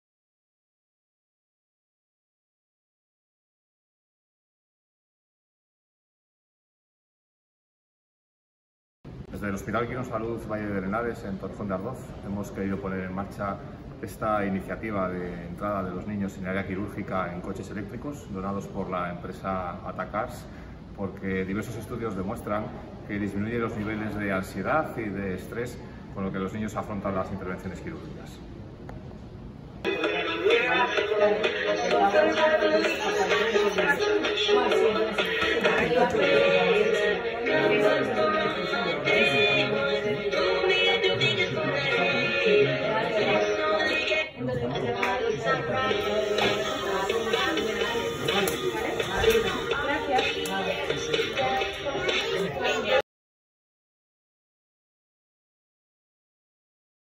Coches eléctricos para cirugía de niños
El Hospital Quirónsalud Valle del Henares ha puesto en marcha un servicio de coches eléctricos, gracias a la donación del Grupo Ataa Cars, para llevar a los niños al quirófano con el fin de reducir la ansiedad y el estrés.